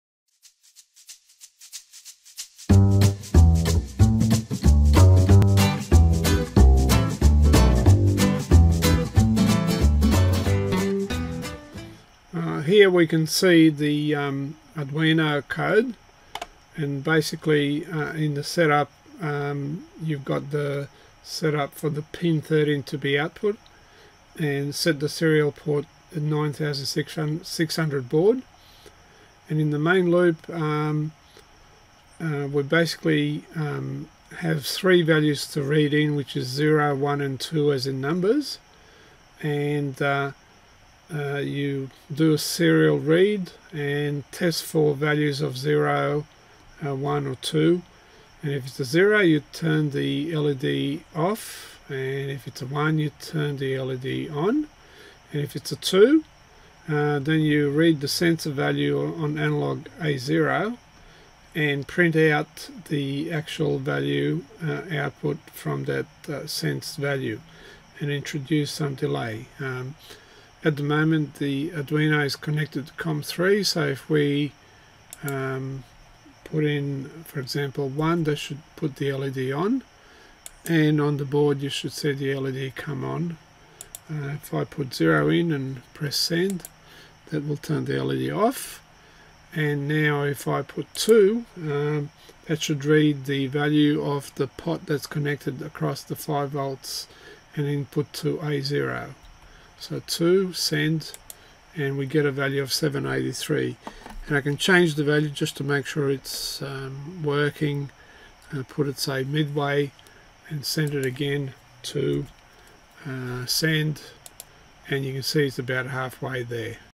Arduino Serial LED control and Analog input
This short video shows you an example of LED BLINK example via Serial port commands. In a similar way use the serial port and read the A0 analog value on demand.